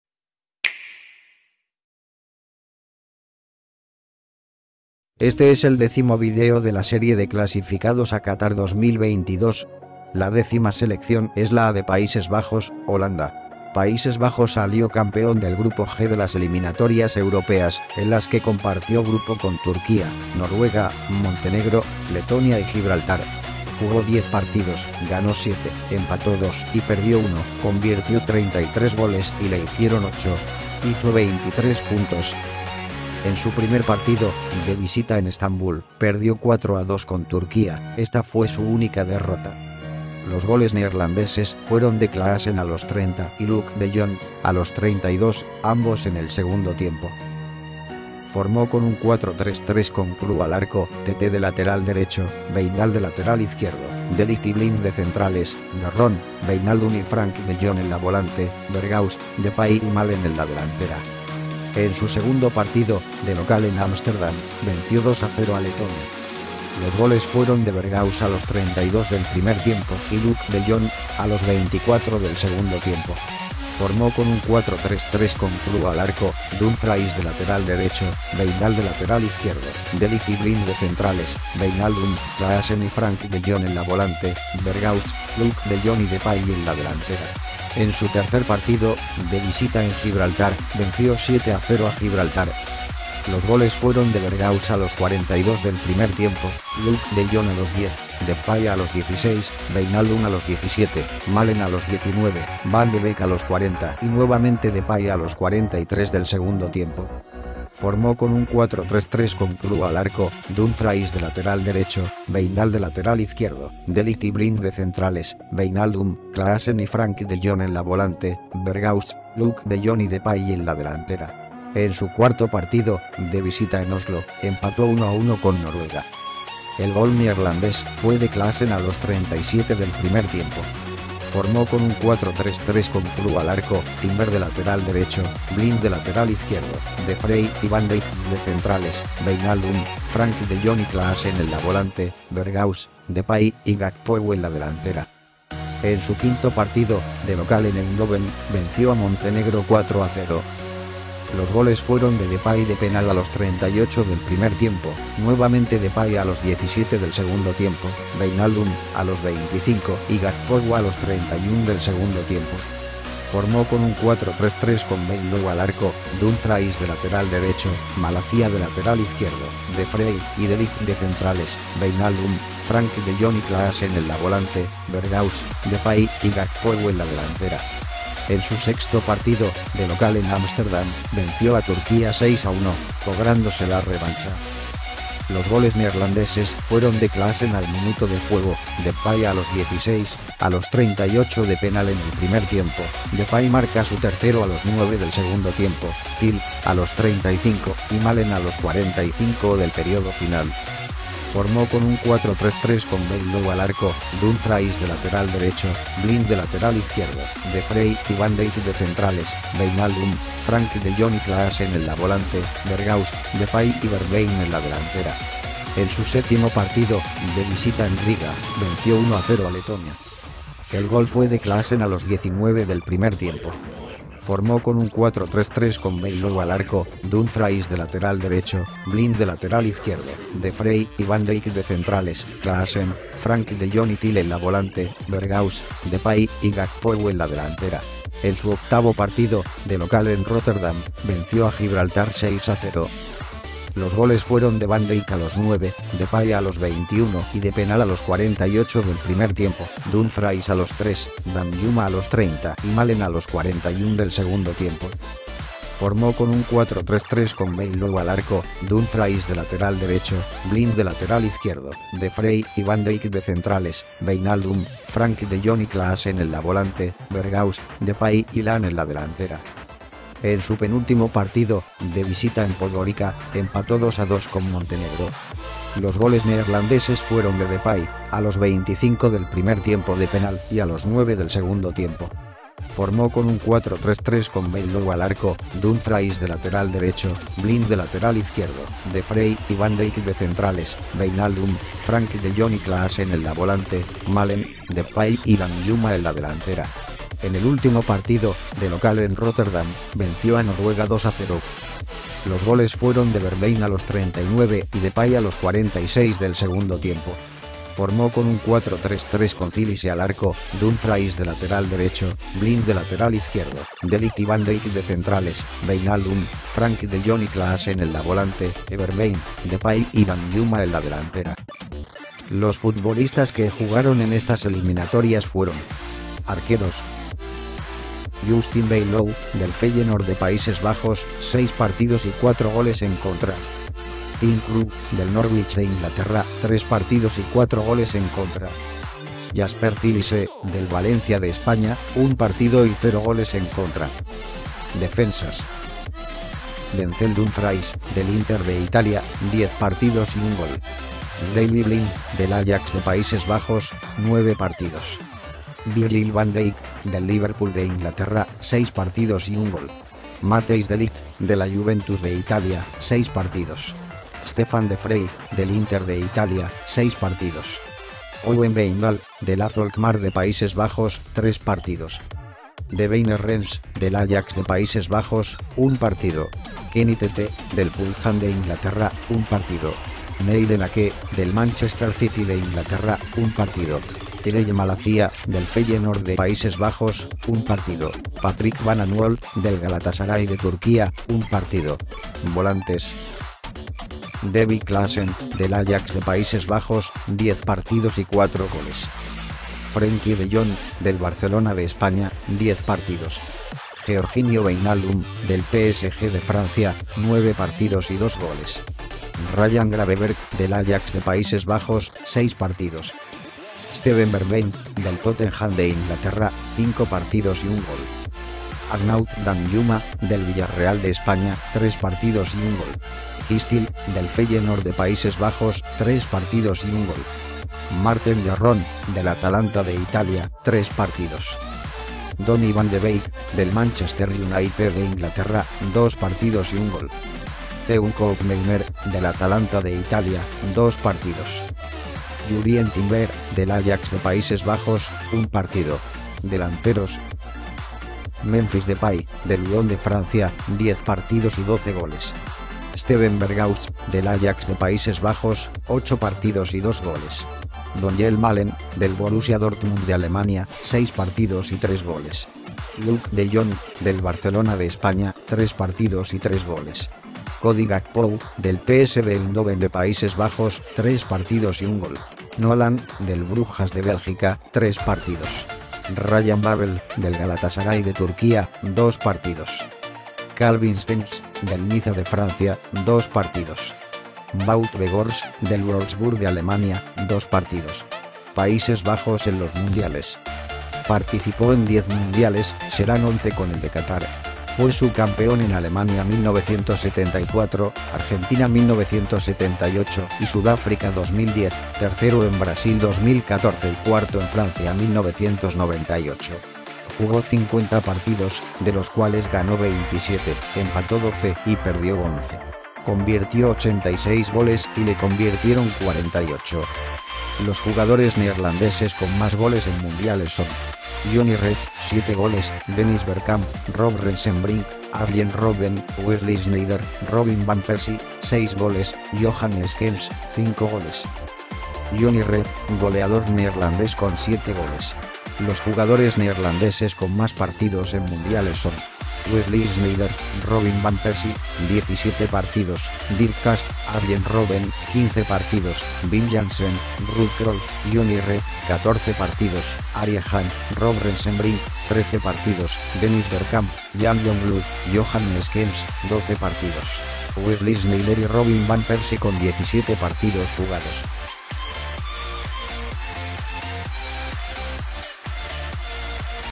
QATAR 🇶🇦 2022 - PAÍSES BAJOS (HOLANDA) 🇳🇱 NEDERLAND 🇳🇱 gekwalificeerd voor QATAR 🇶🇦 2022
Países Bajos 🇳🇱 clasificó a Qatar 2022 y será su participación número 11.

Países Bajos salió campeón del Grupo G de las Eliminatorias Europeas, en las que compartió grupo con Turquía, Noruega, Montenegro, Letonia y Gibraltar.

Blog de trapo.ver.pe (sin espacios)